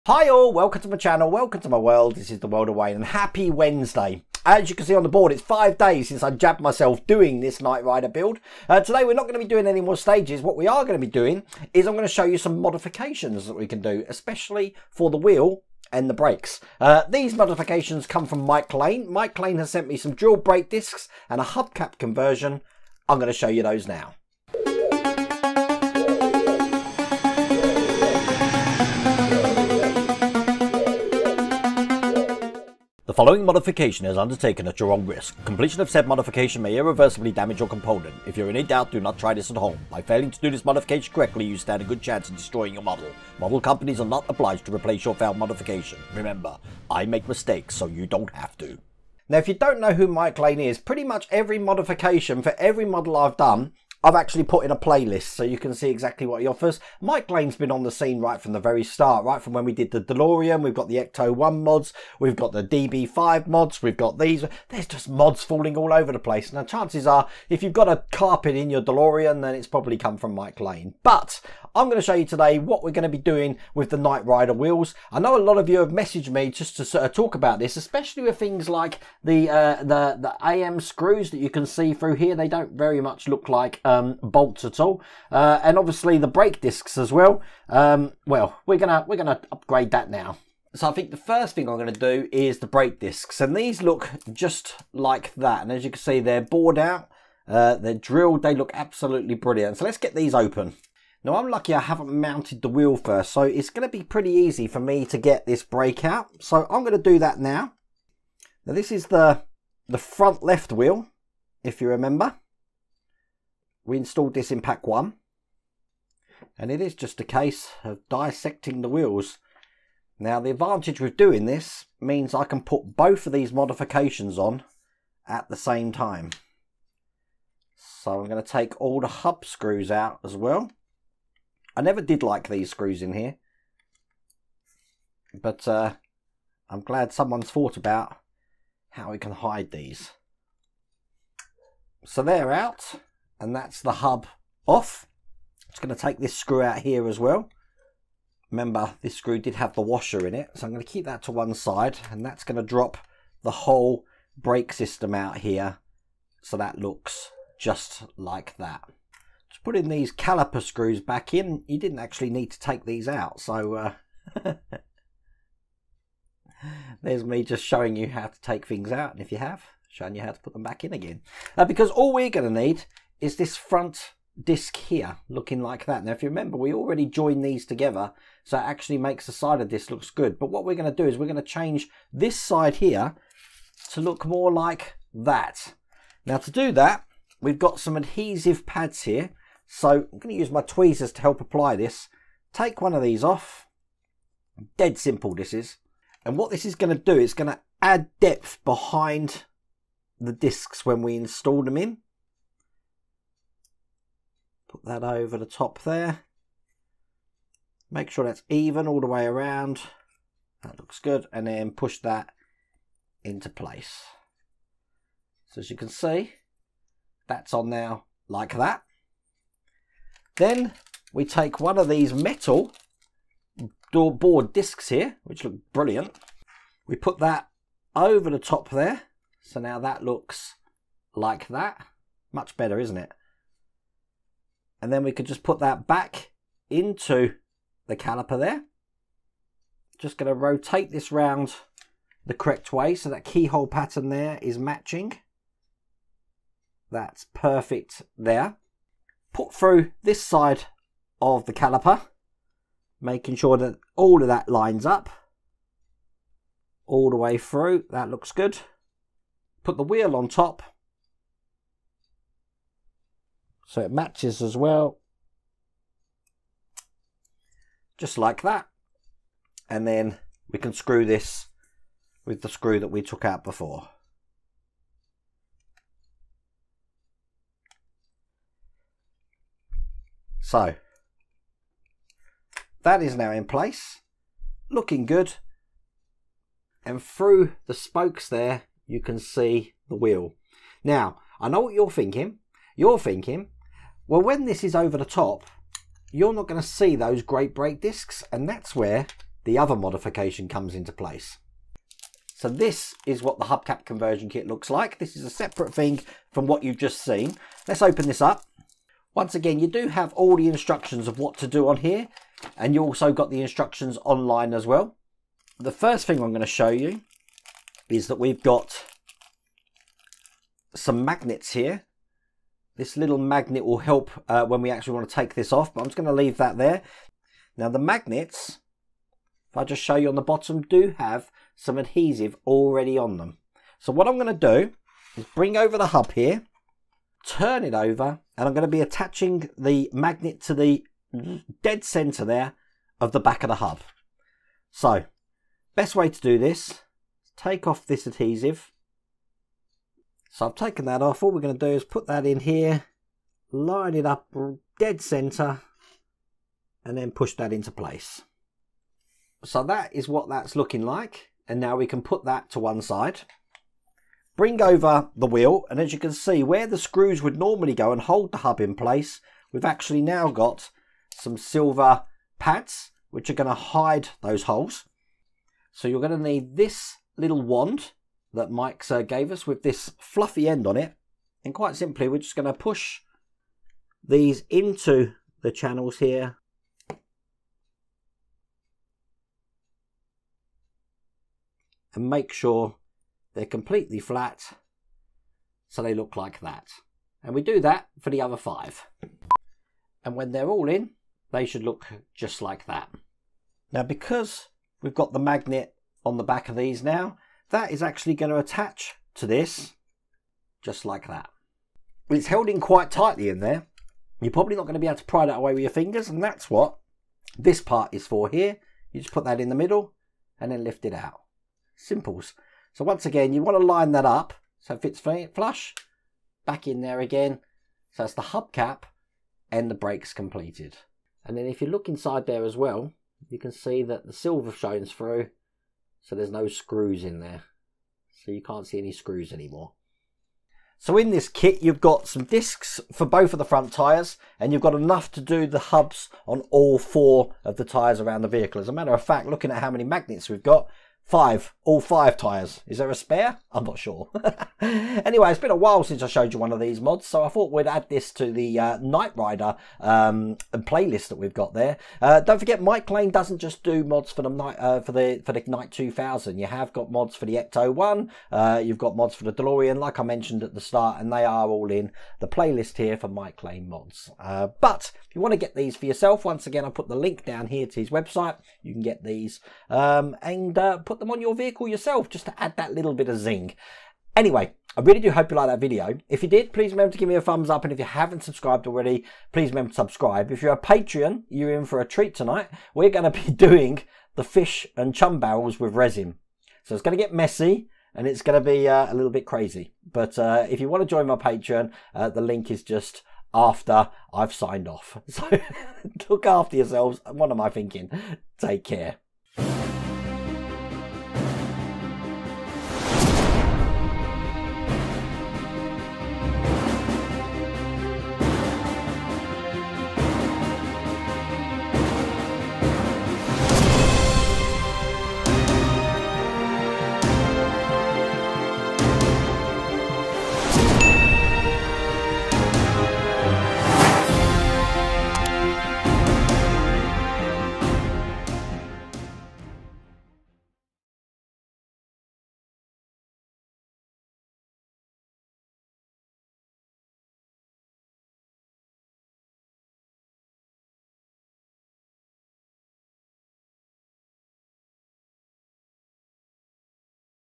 Fanhome Build the Knight Rider KITT - Mike Lane Mods - Drilled Brake Discs and Hub Cap Conversion Ki
This is a 1:8 scale model car from the hit series "Knight Rider". You can get this collection from

If you would like the shirt I am wearing in this build series head on over to